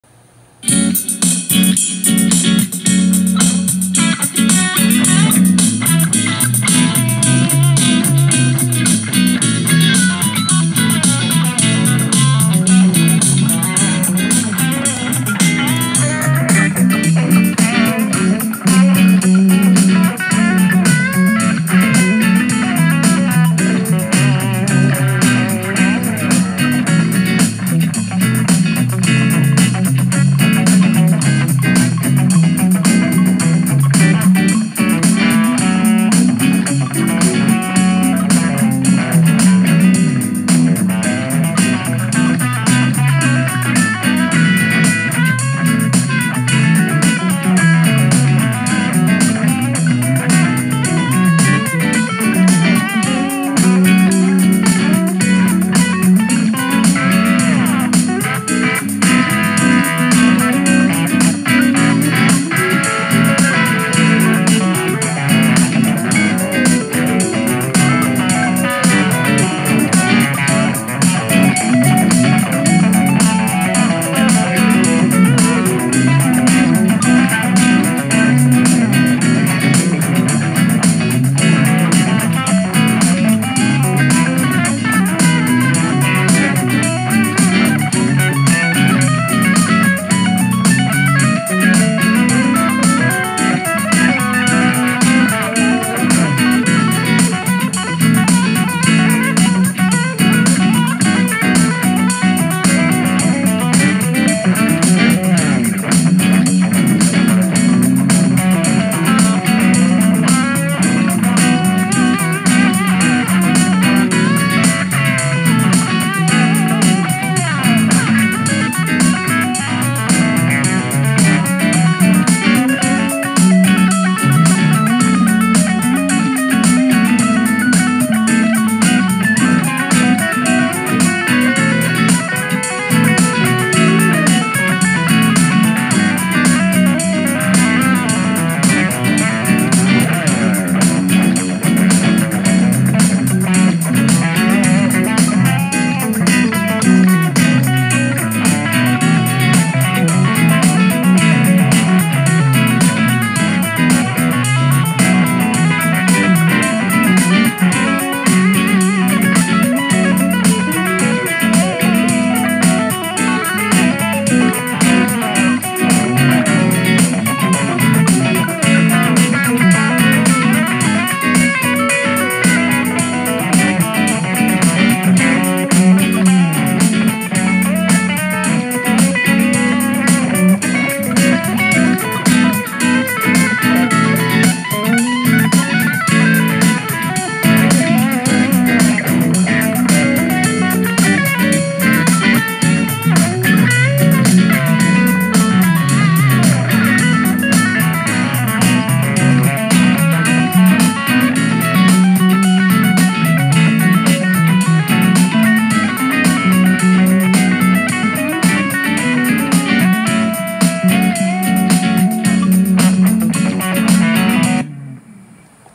The I haven’t played my guitar in 4+ months jam.
Throwing down some delicious burnt cheese jam goo. Too fun to even describe.